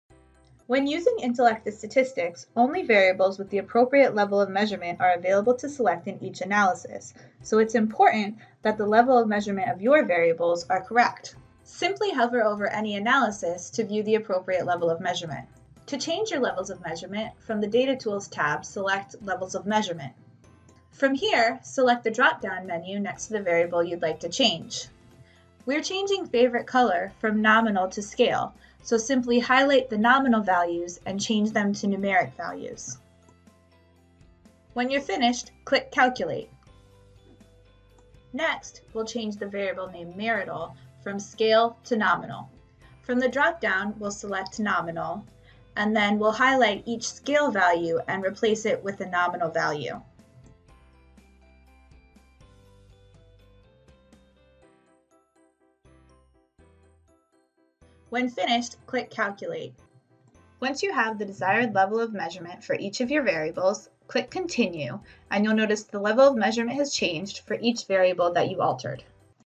Change Level of Measurement
How to change your level of measurement using Intellectus Statistics. We change a nominal variable to scale, and a scale variable to nominal.